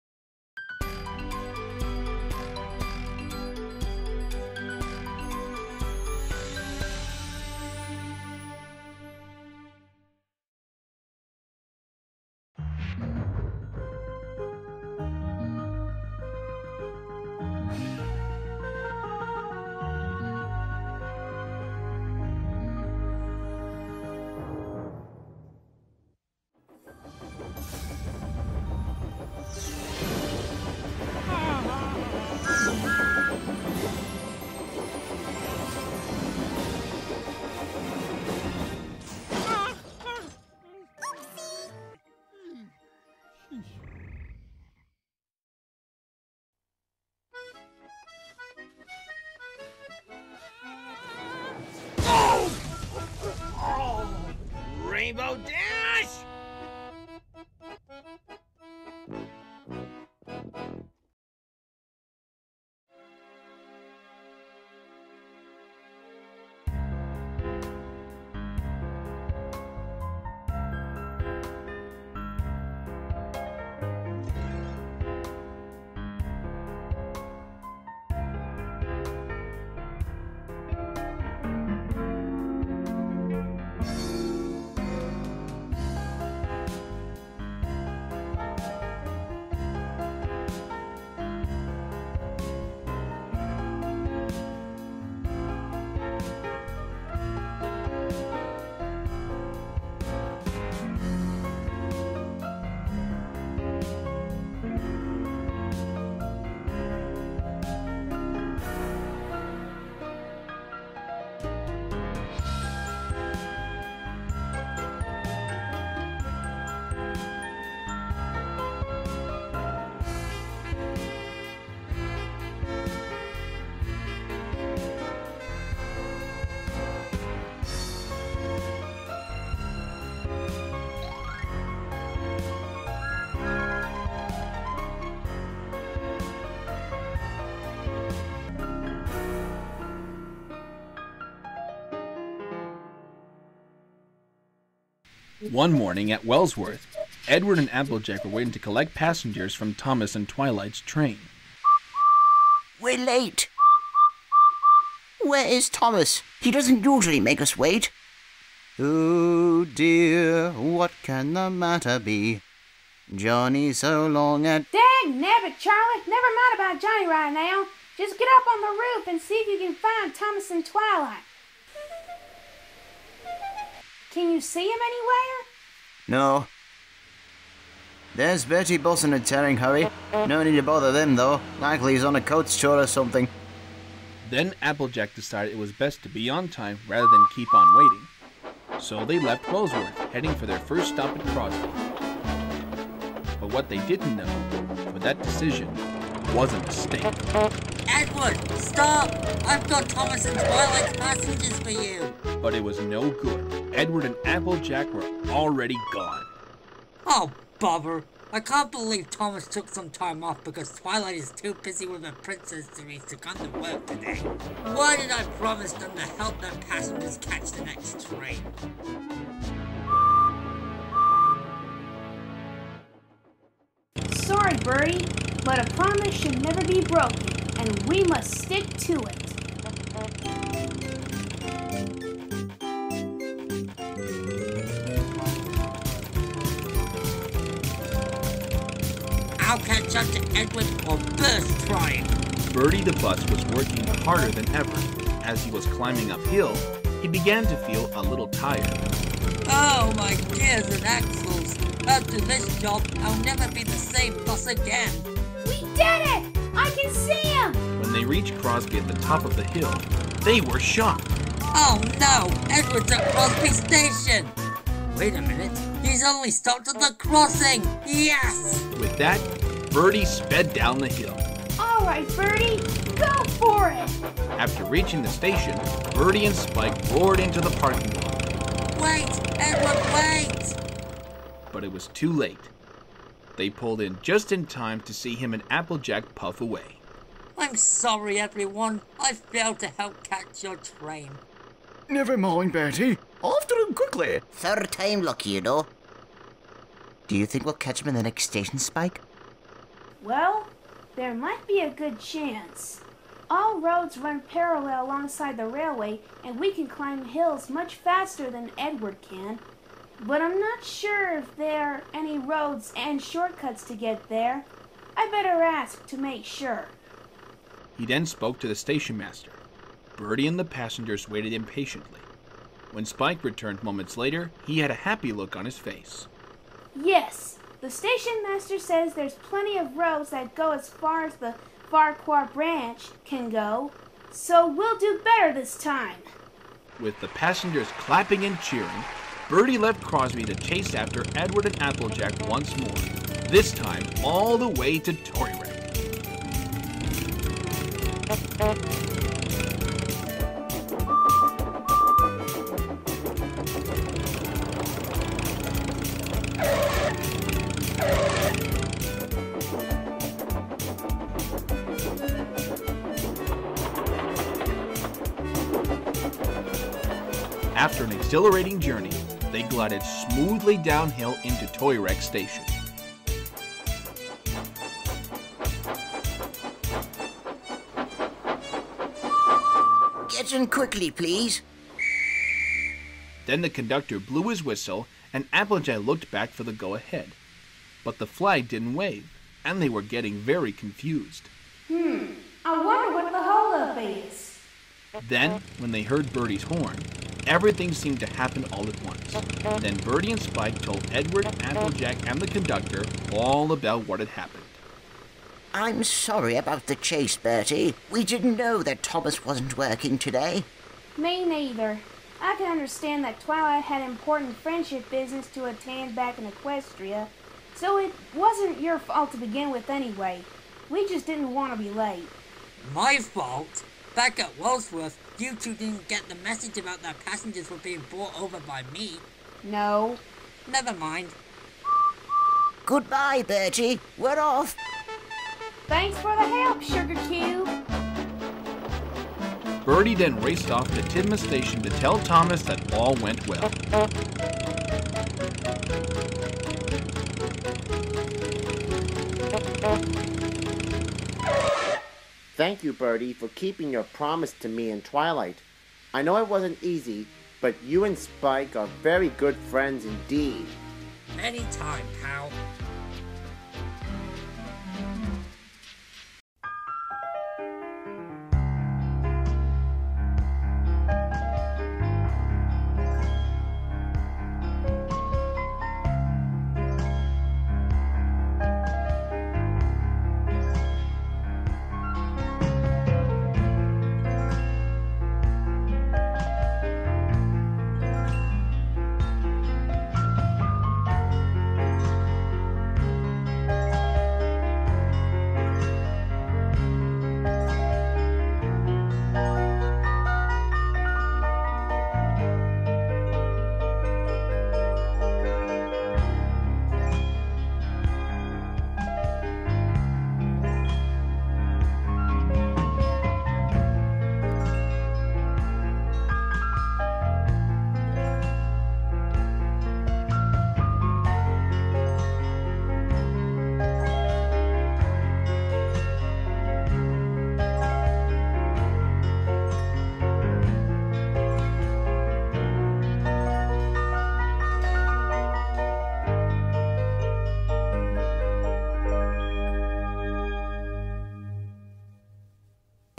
Sodor Pony Railway Adventures Season 2 Episode 3-Edward & Bertie
Hey everypony! After a hiatus of 2 months, Sodor Pony Railway Adventures is back with another new episode for your enjoyment. In this adventure, Bertie & Spike chase after Edward & Applejack when they left Wellsworth Station after getting impatient of waiting for Thomas & Twilight to bring their passengers but didn't show up. Will Bertie & Spike be able to delivered their passengers to Edward & Applejack before it's too late? Find out in the 1st story of the Edward/Applejack trilogy. Also, Happy 4th of July everypony!! Thomas & Friends & My Little Pony belong to HIT Entertainment & Hasbro Studios.